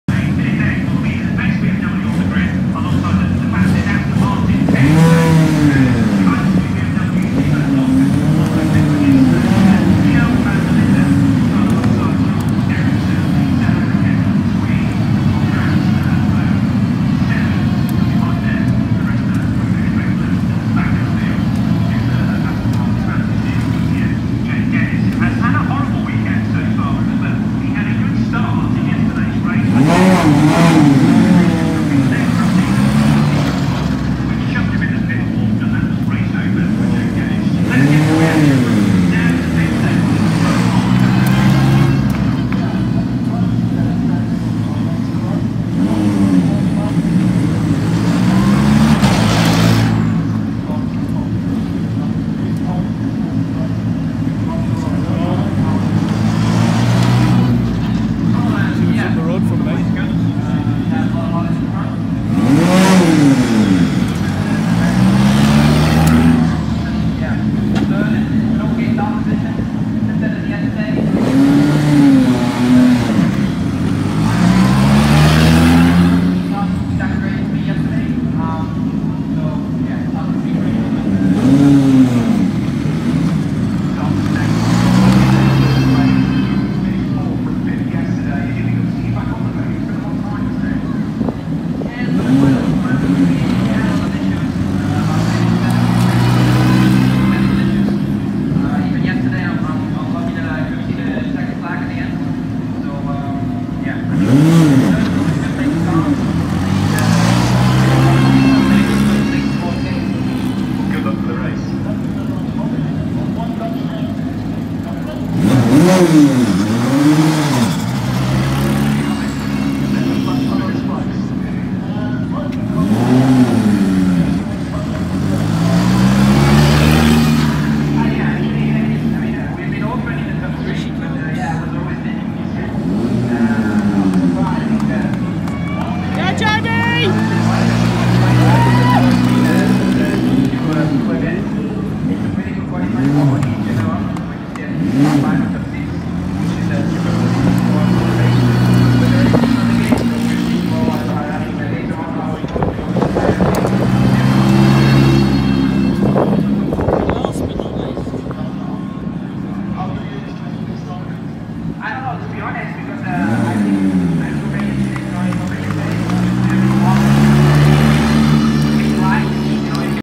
W Series - Heading our for Quali
The cars leaving the support paddock to head out for qualification Sunday 11th August 2019